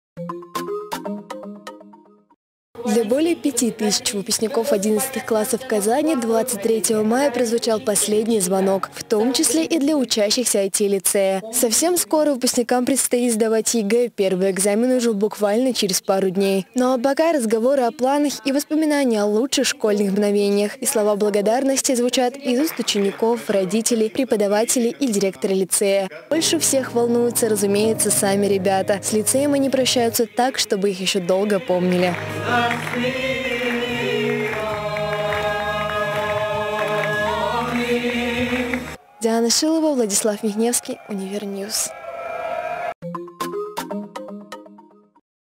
Взрослая жизнь сменила школьные грезы
В IT-лицее КФУ прозвучал последний звонок, и одиннадцатиклассники сделали первый шаг во взрослую жизнь.